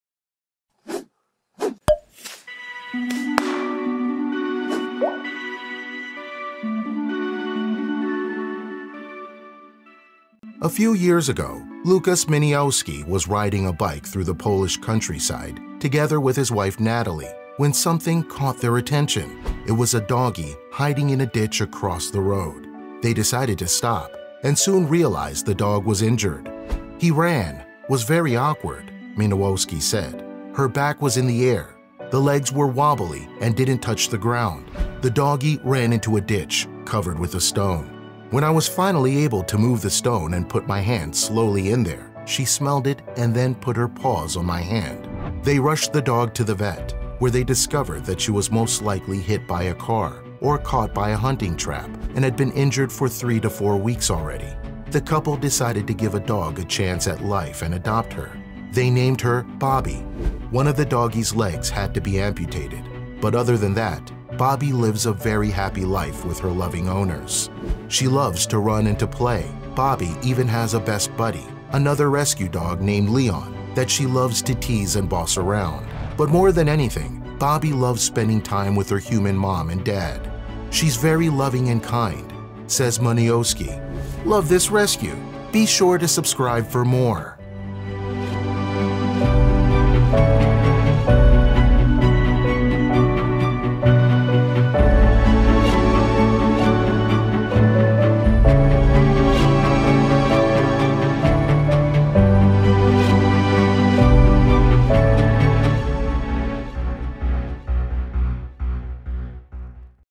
This Dog With Broken Legs Lived All By Herself In A Ditch — Until Someone Saved Her
A few years ago, Łukasz Muniowski was riding a bike through the Polish countryside together with his wife Natalia, when something caught their attention. It was a doggie hiding in a ditch across the road.

They decided to stop, and soon realized the dog was injured. “Her run … was very awkward,” Muniowski said. “Her back was in the air, the back legs were wobbly and didn’t touch the ground.”

The doggie ran into a ditch covered with a stone. “When I was finally able to move the stone and put my hand slowly in there… she smelled it and then put her paws on my hand.”

They rushed the doggie to the vet, where they discovered that she was most likely hit by a car or caught by a hunting trap, and had been injured for 3-4 weeks already.

The couple decided to give a dog a chance at life and adopt her. They named her Bobby.

One of the doggie’s legs had to be amputated, but other than that, the dog lives a very happy life with her loving owners. She loves to run and to play. Bobby even has a best buddy, another rescue dog named Leon that she loves to tease and boss around. But more than anything, Bobby loves spending time with her human mom and dad. “She’s very loving and kind,” says Muniowski.